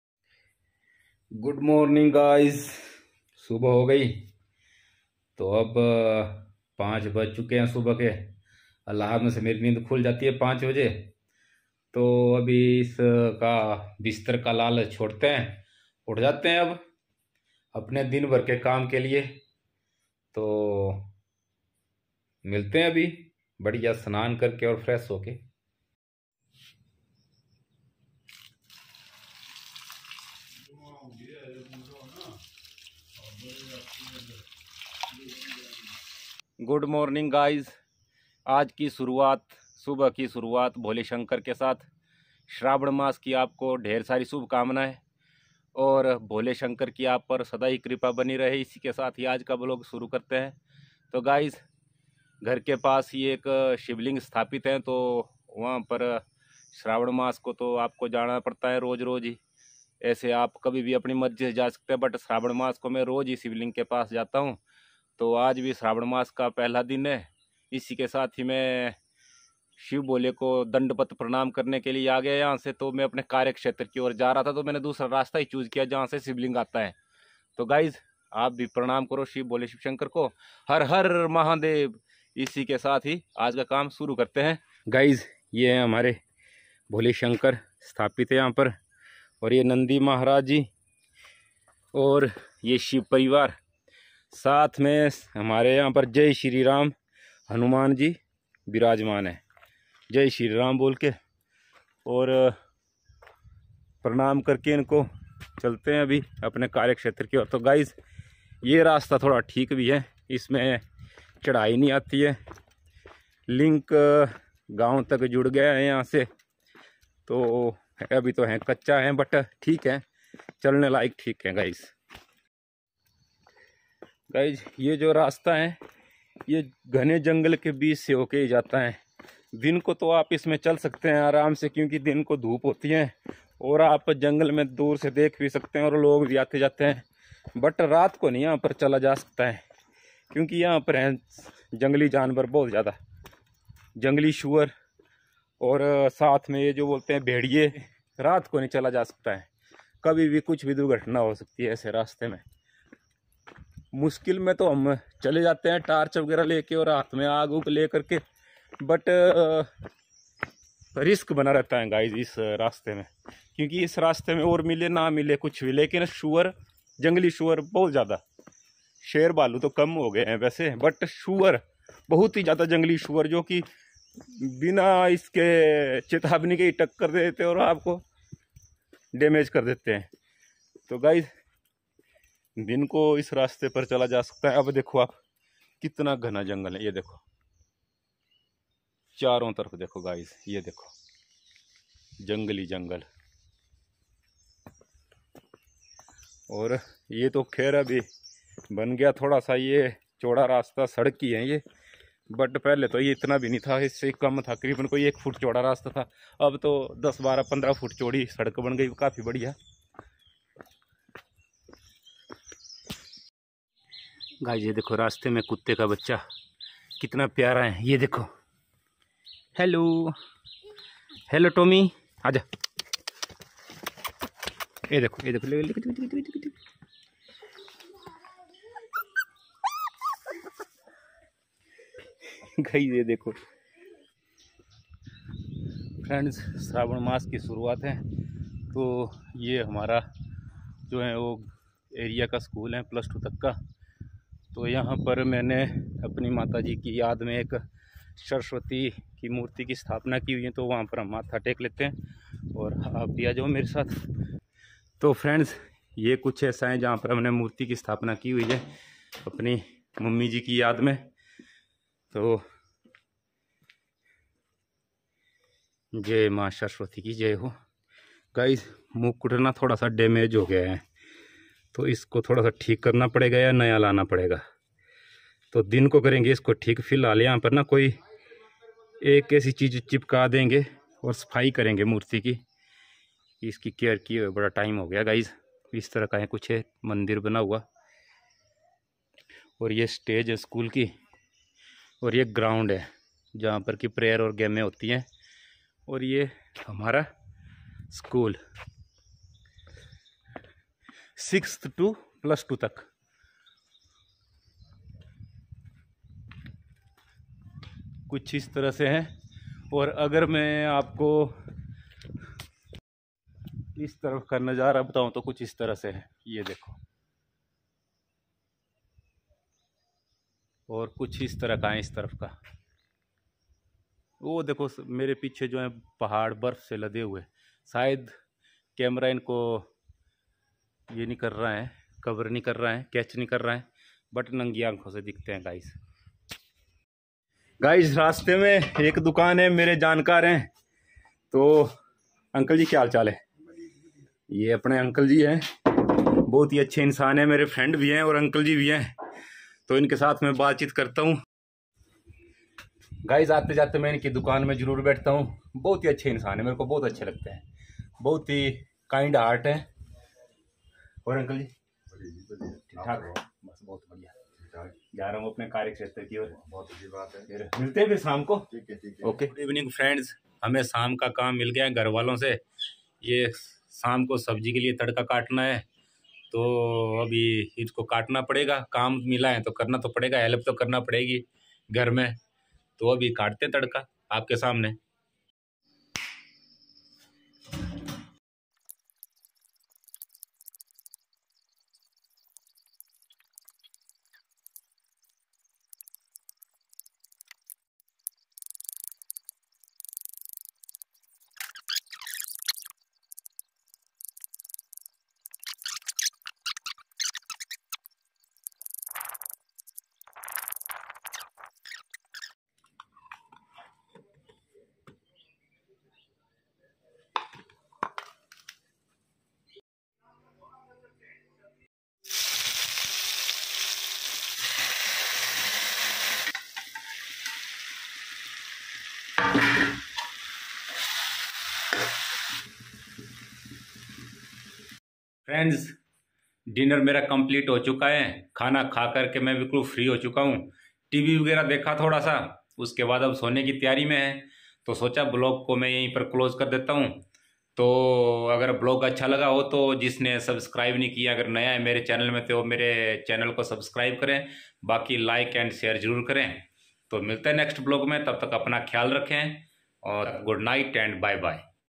सावन महीने की आप सभी को शुभकामनाएं ||HAPPY SAVAN MONTH TO ALL OF YOU||the royal villager 2.O
सावन महीने की आप सभी को शुभकामनाएं ||HAPPY SAVAN MONTH TO ALL OF YOU||the royal villager 2.0

friends is video mein  pahle to Shiv Mandir mein Gaya vahan per shivling ke aage aur Shiv Parivar ke aage matha teka uske bad maine aapko bataya ki mere gaon ka rasta kaisa hai sadak kaisi hai uske bad mere ko raste mein ek doggy ka baccha mil Gaya tha aur friendly tha uske bad mein Mandir Gaya Jo banaya hua hai Maine apni Mummy ki yad mein Apne area ke higher secondary school mein . uske bad mere ko Mera dost mil Gaya aur bade dinon bad mein uski dukaan mein Gaya